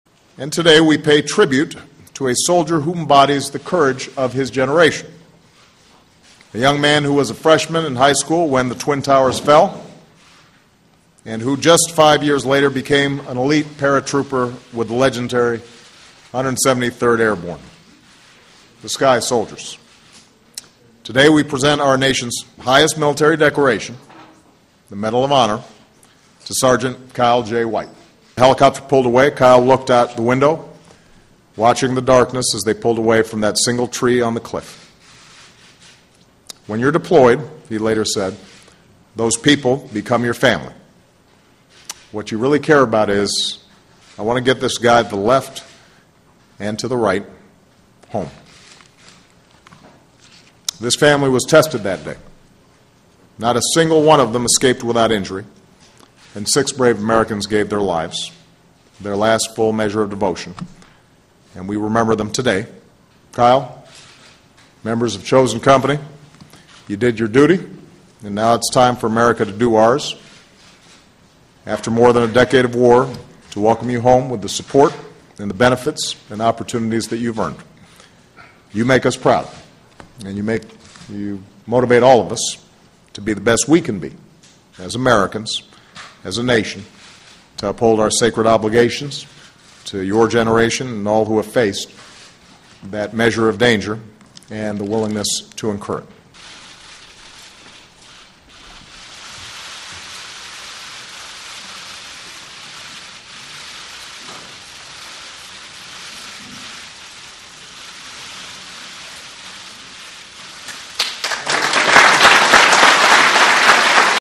Afghanistan Veteran Receives Top Medal at WH
President Barack Obama has presented the Medal of Honor to former Army Sgt. Kyle White for heroism during an ambush in Afghanistan in 2007. White saved a comrade's life and helped ensure that other wounded survivors were evacuated. (May 13)